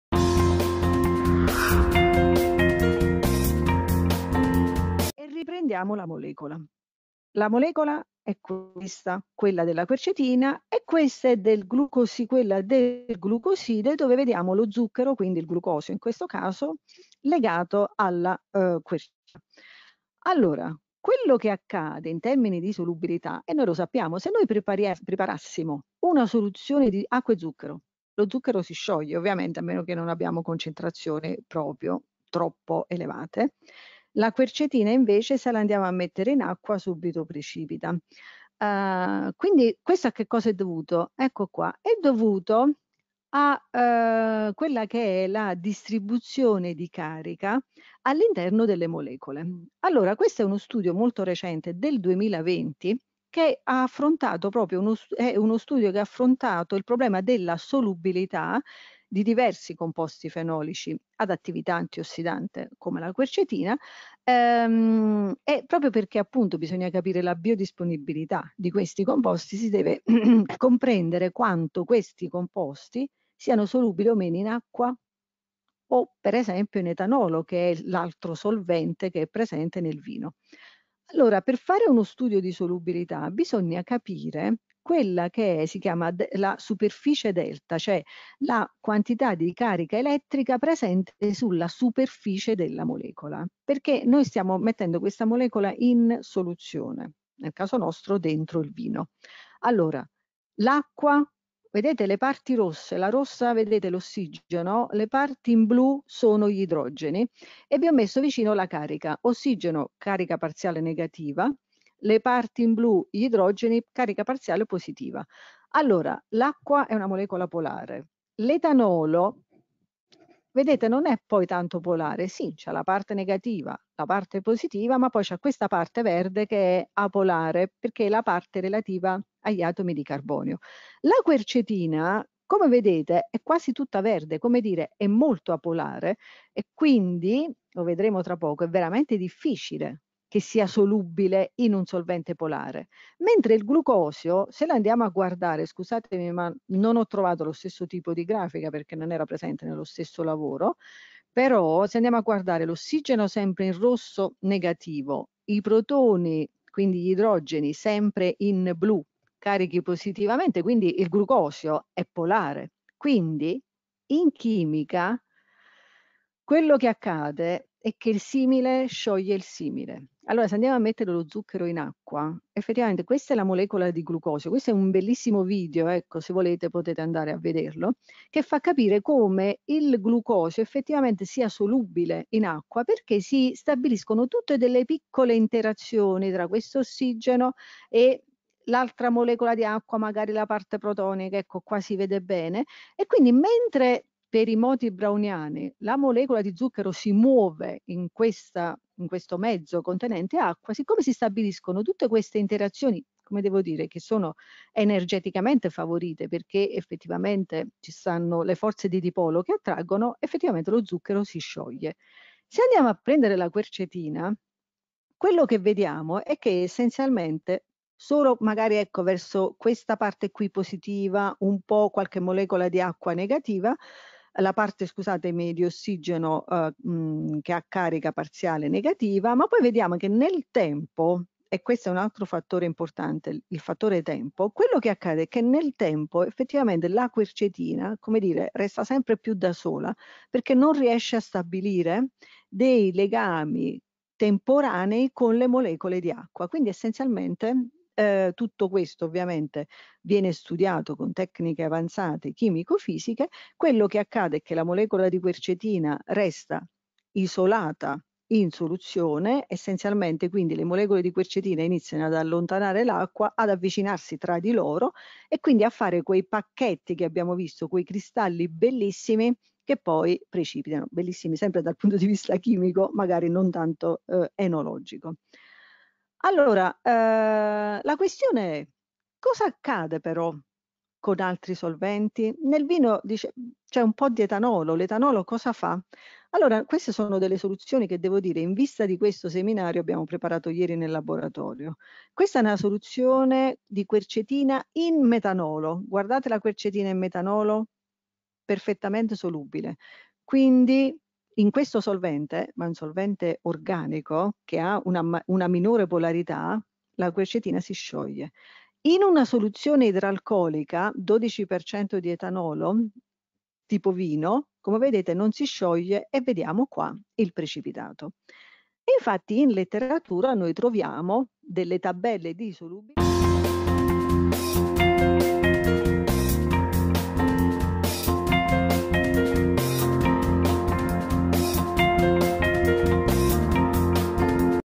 La solubilità della Quercetina nei vini
Relatore: Angelita Gambuti, Università degli Studi di Napoli Federico II

Questo video è stato estratto da un webinar organizzato in collaborazione tra Laffort e Vinidea, puoi accedere alle registrazioni integrali qui

La problematica inerente alle precipitazioni di quercetina aglicone nei vini non pare essere di facile soluzione vista la complessità dei meccanismi biochimici coinvolti.
Le pratiche agronomiche volte ad incrementare le concentrazioni dei succhi, le tecniche enologiche conservative ed il riscaldamento climatico sembrano accentuarne l’incidenza.
Da qui il nostro impegno nello studio del suo comportamento ed evoluzione nel vino per arrivare a proporre una possibile soluzione.